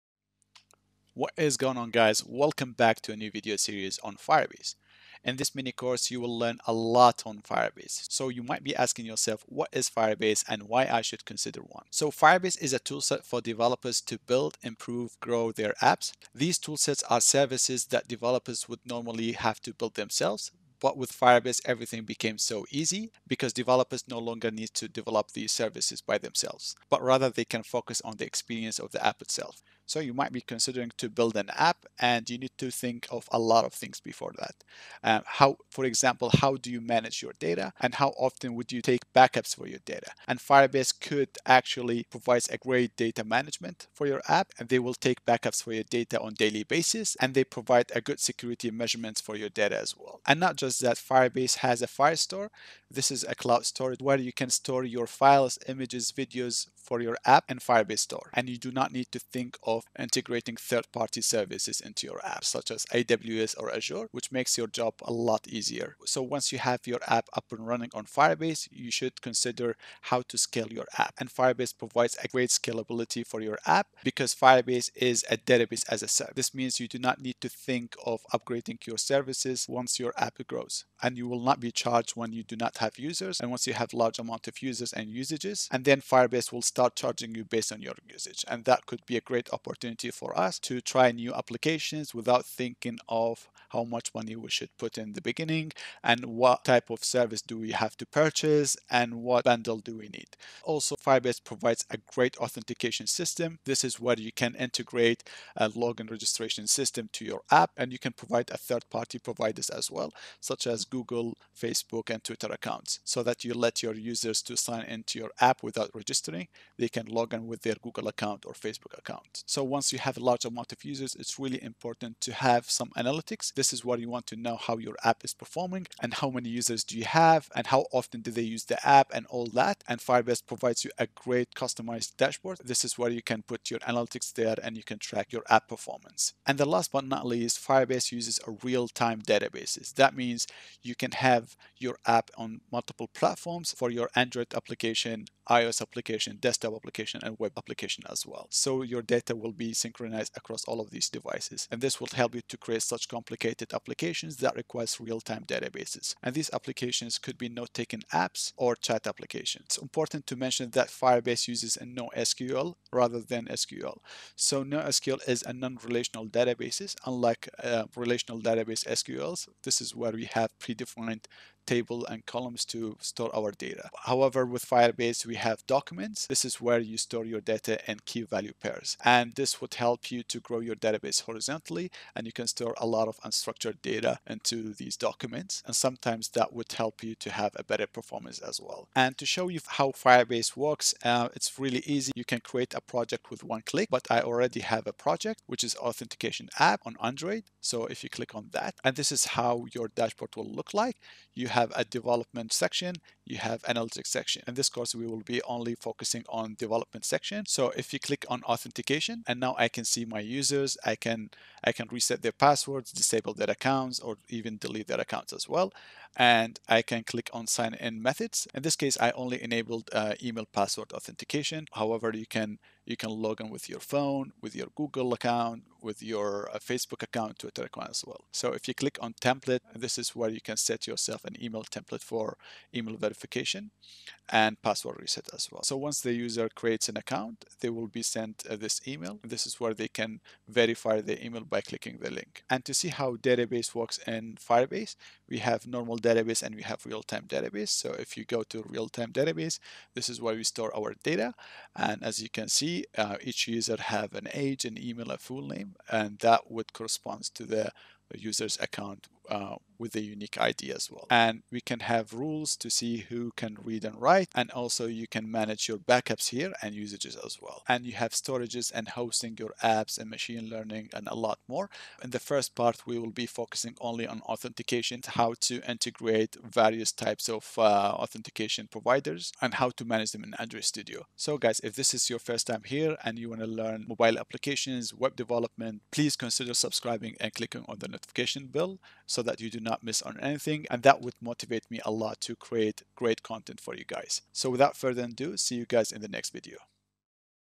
#0 What is Firebase ?
There are many questions that I will address in this video such as what is Firebase? What is Firebase used for? Who Can use Firebase?
Firebase is a complete back-end solution that can manage authentication, real time database and hosting as well. You can use Firebase in iOS apps, android apps and even in web apps. For more watch this video..